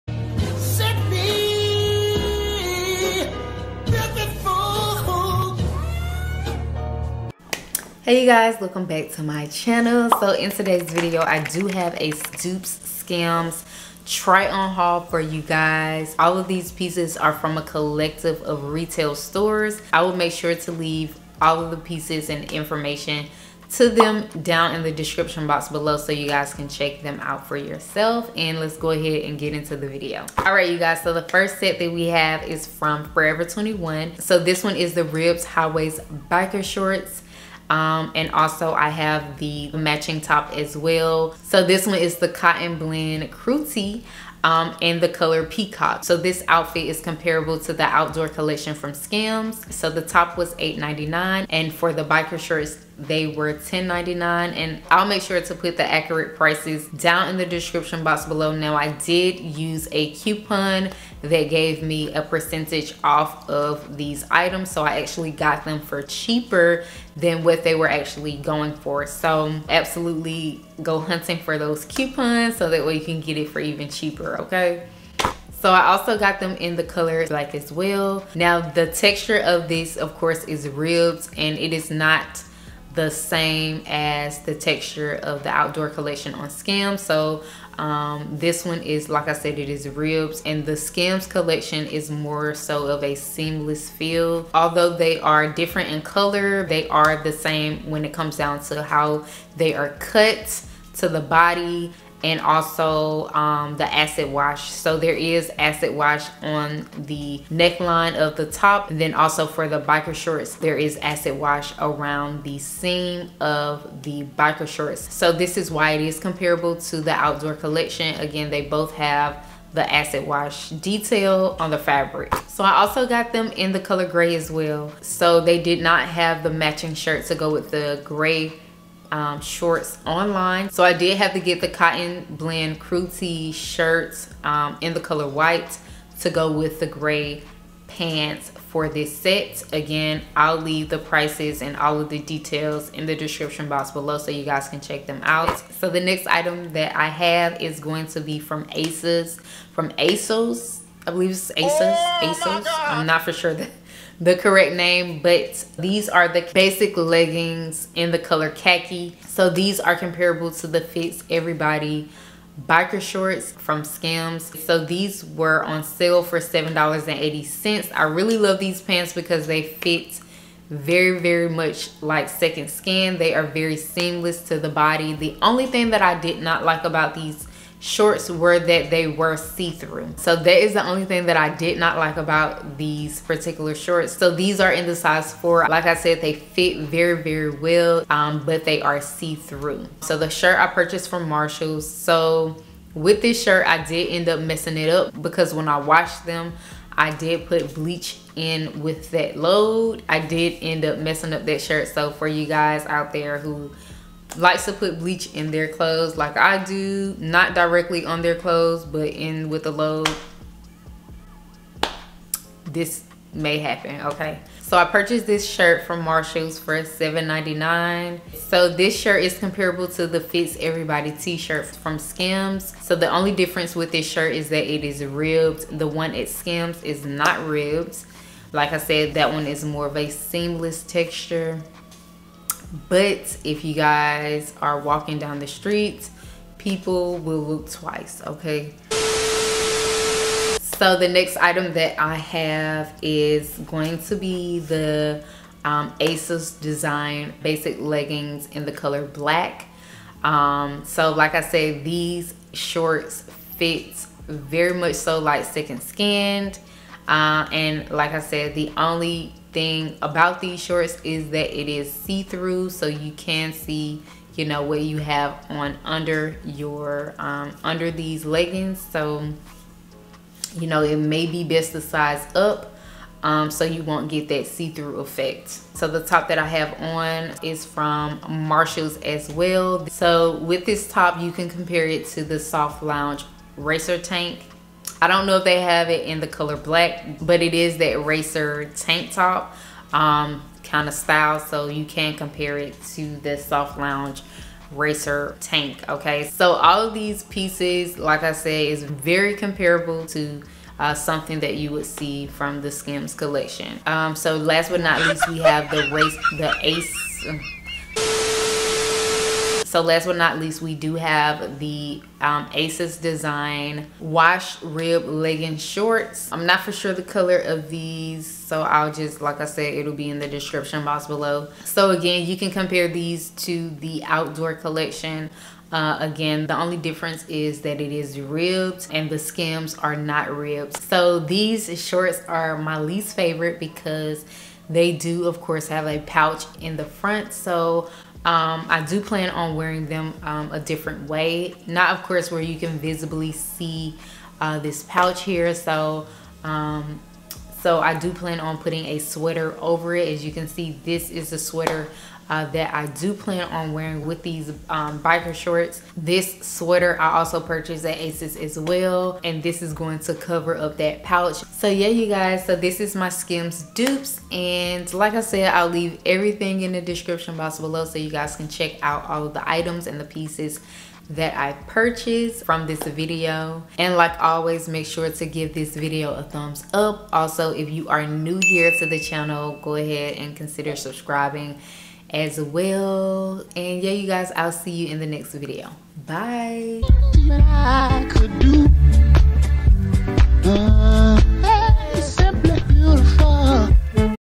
The Best In QUALITY *very affordable* Skims Dupes Try On Haul
The Best In QUALITY AND affordable*  Skims Dupes Try-On Haul YOU'LL EVER SEE ON YOUTUBE!

MAKE SURE TO BE A PART OF THE CHANEL BY BECOMING A SUBSCRIBER AND
ALSO

HERE ARE THE LINKS TO ALL OF THE ITEMS ❤️
ASOS x FOREVER 21 x MARSHALs

*All shirts from Marshalls were purchased at stores*